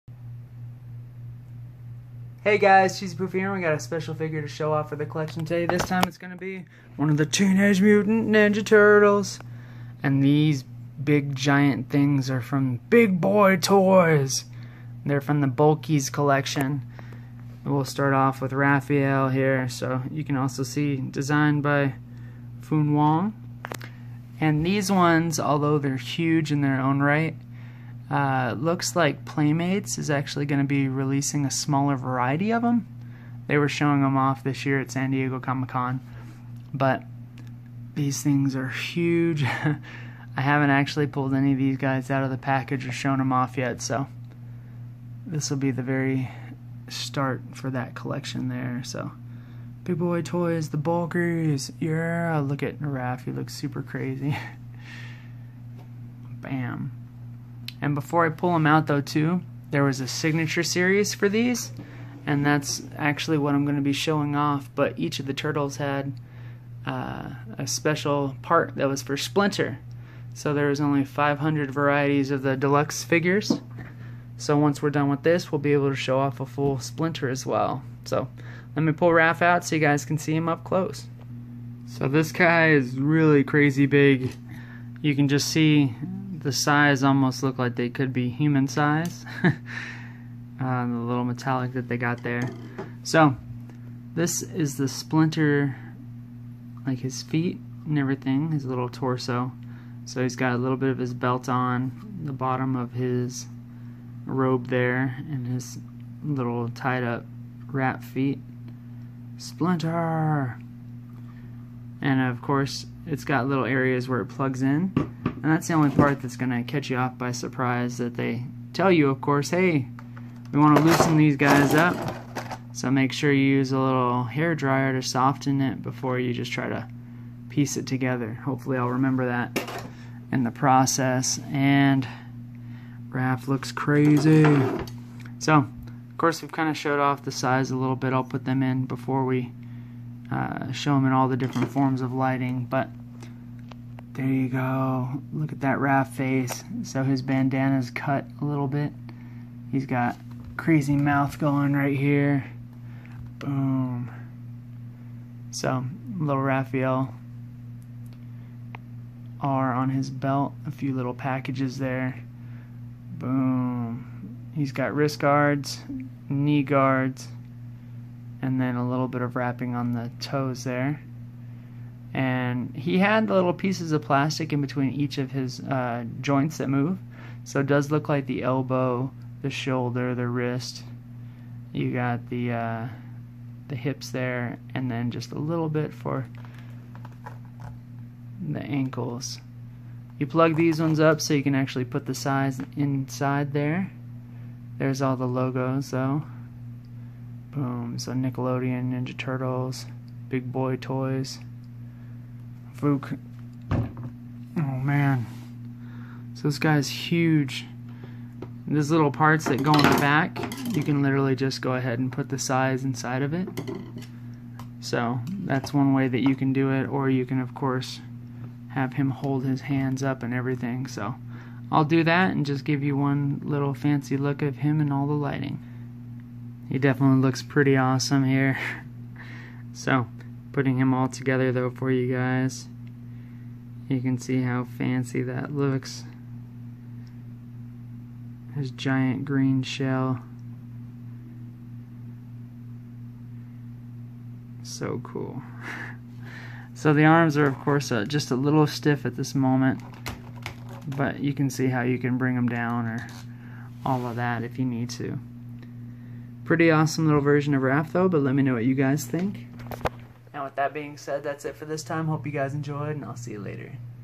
TMNT Raphael Bulkyz Bigboystoys Ninja Turtles Figure Review
Raphael Bulkyz